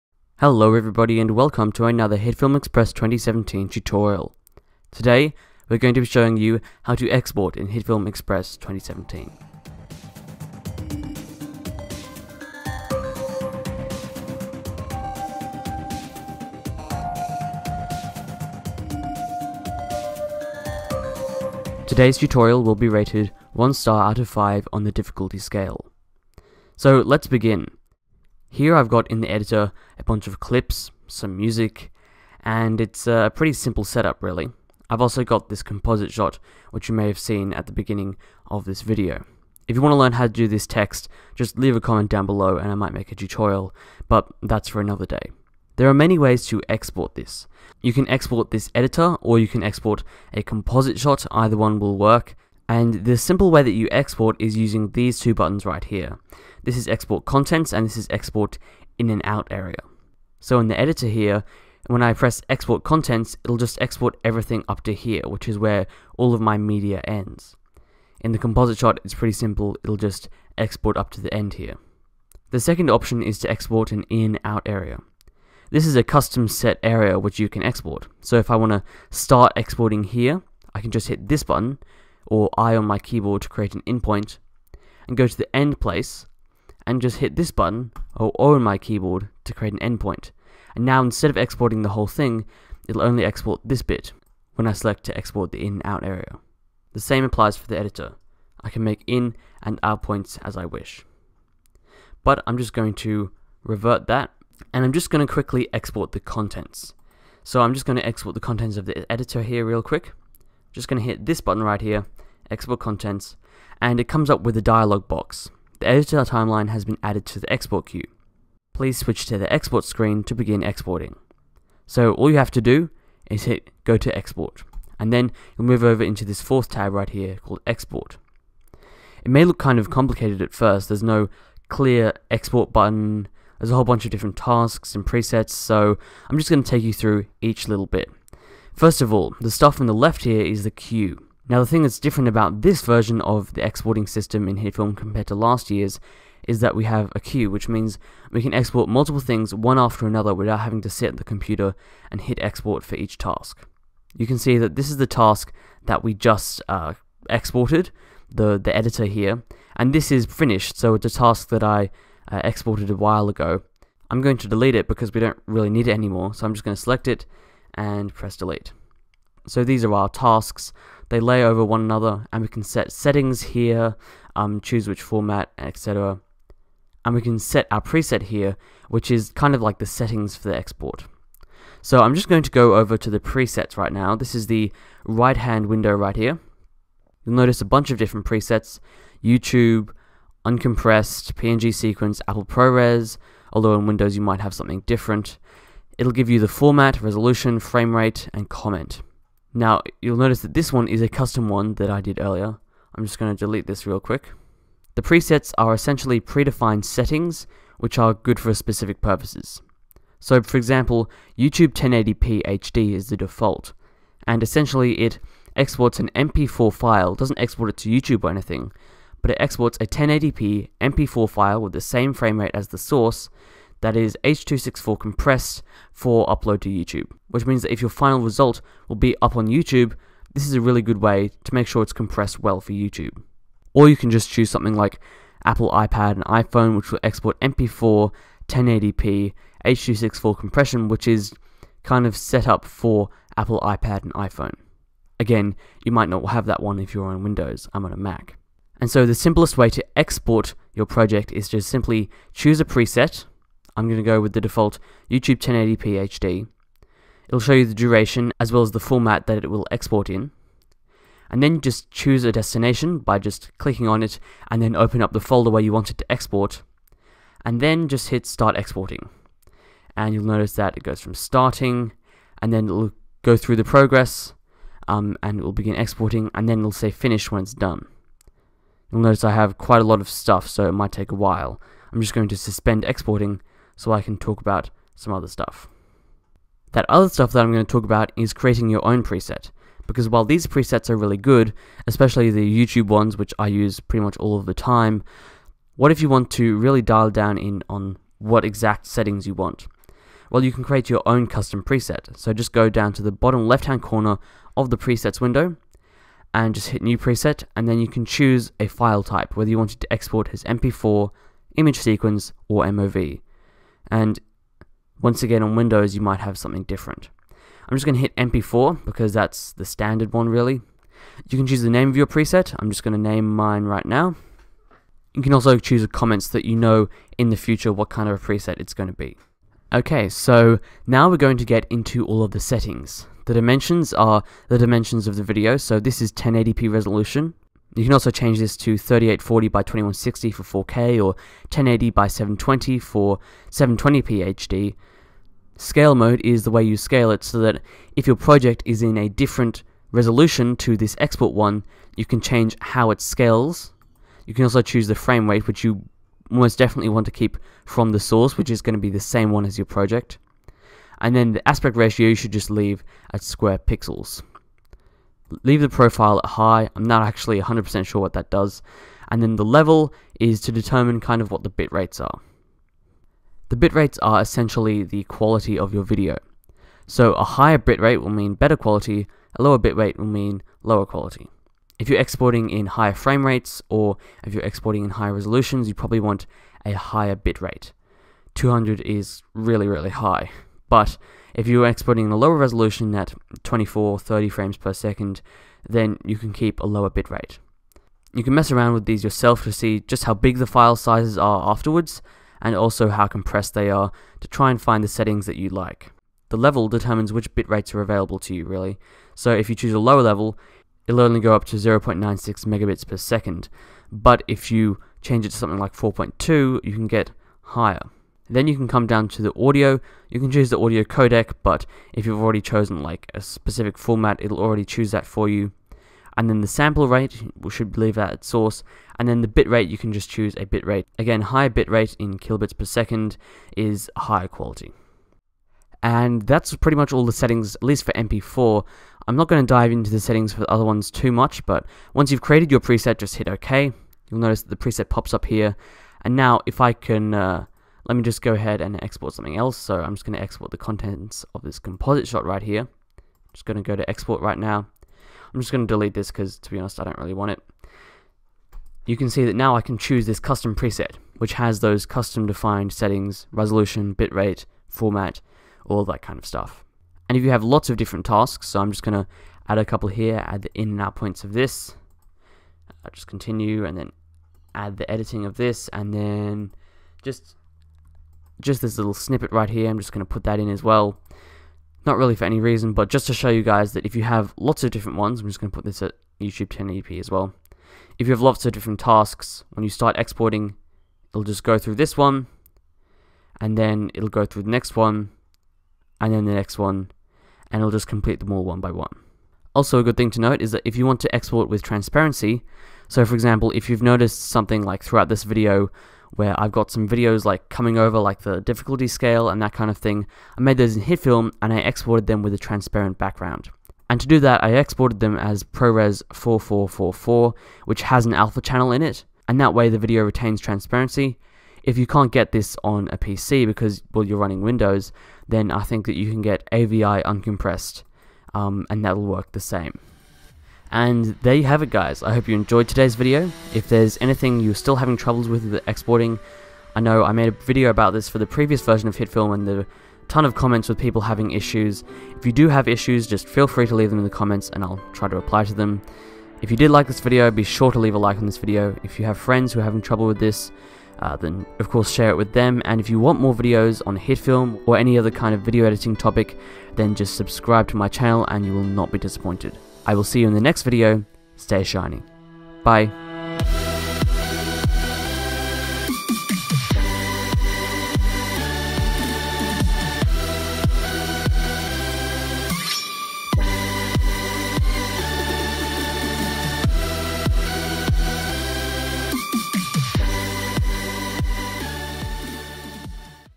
Hitfilm Express 2017 - How to Export (Export Queue)
Hitfilm Express 2017 has a rather complicated export system. However, with such a system you can fully customize your export settings and export multiple files one after another. MP4, MOV, AVI, and Image sequence can all be exported in Hitfilm, as well as 60fps and 4K video.